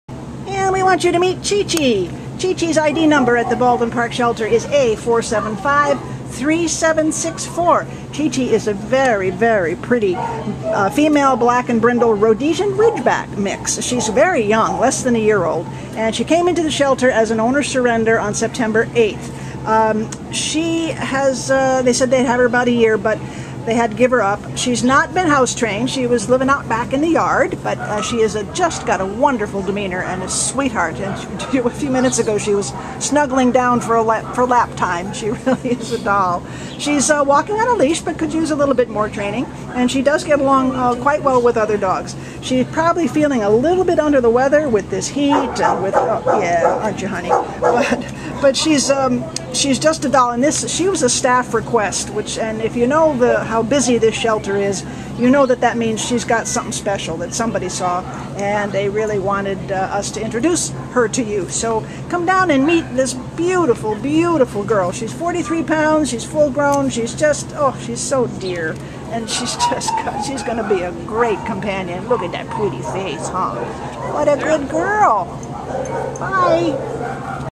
A4753764 Chichi
A4753764 Chichi is a majestic 7-month-old black-and-brindle female Rhodesian Ridgeback and Belgian Malinois mix puppy who was left at the Baldwin Park Animal Care Center with Winston [A4753769] on September 8th with no reason given. Weighing 43 lbs, Chichi has earned a spot among the shelter staff as a favorite! This beautiful girl walks well on leash and has an affectionate and calm manner for a puppy. She also gets along very well with other dogs her size. We think Chichi will make a devoted indoor pet and companion for an active individual or family living in a private home.